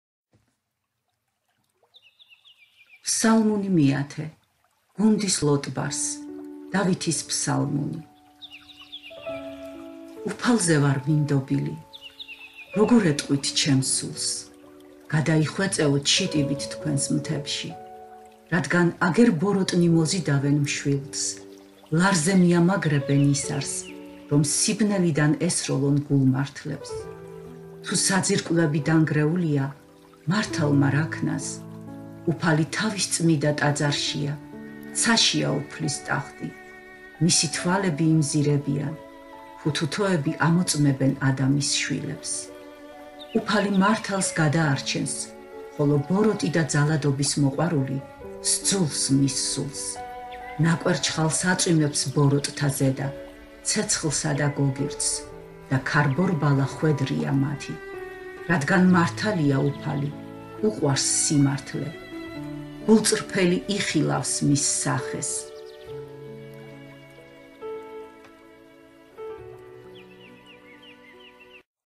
ფსალმუნი 10
ფსალმუნი 10 - კითხულობს მარიკა ჭეღელიძე.